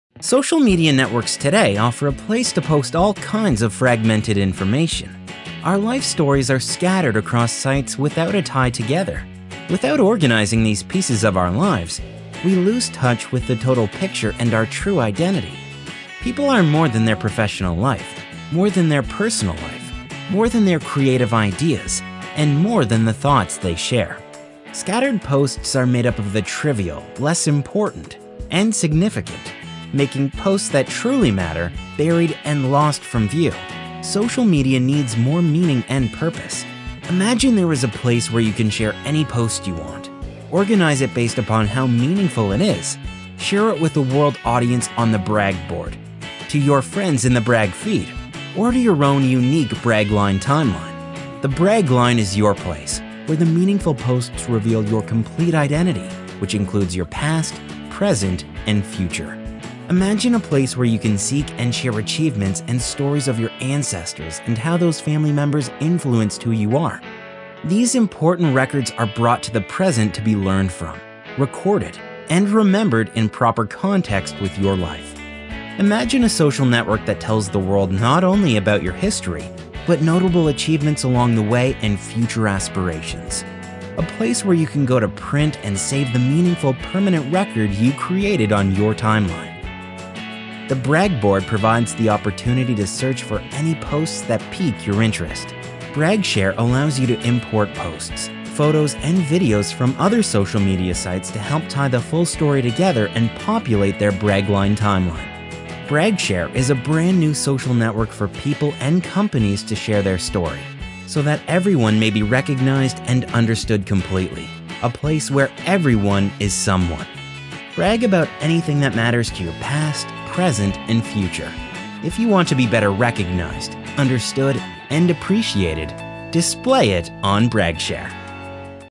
Whiteboard Doodle Animation - Animated Explainer Video (Bragshare.com)
Animated explainer video created for social media network Bragshare.com

Script transcript is below:

"Social media networks today offer a place to post all kinds of fragmented information. Our life stories are scattered across sites without a tie together. Without organizing these pieces of our lives, we lose touch with the total picture and our true identity. People are more than their professional life, more than their personal life, more than their creative ideas, and more than the thoughts they share.
Scattered posts are made up of the trivial, less important, and significant, making posts that truly matter, buried and lost from view. Social Media needs more meaning and purpose.

Imagine there is a place where you can share any post you want, organize it based upon how meaningful it is, share it with a world audience on the Bragboard, to your friends in the Bragfeed, or to your own unique Bragline timeline. The Bragline is your place, where the meaningful posts reveal your complete identity, which includes your past, present, and future.
Imagine a place where you can seek and share achievements and stories of your ancestors and how those family members influenced who you are. These important records are brought to the present to be learned from, recorded, and remembered in proper context with your life.

Imagine a social network that tells the world not only about your history, but notable achievements along the way and future aspirations.
A place where you can go to print and save the meaningful permanent record you created on your timeline.
A place to go discover all kinds of achievements and stories about other’s lives. The Bragboard provides the opportunity to search for any posts that pique your interest.
Bragshare allows you to import posts, photos and videos from other social media sites to help tie the full story together and populate their Bragline timeline.

Bragshare is a brand new social network for people and companies to share their story, so that everyone may be recognized and understood completely. A place Where Everyone is Someone.
Brag about anything that matters to your past, present, and future
If you want to be better recognized, understood, and appreciated

Display it on Bragshare!"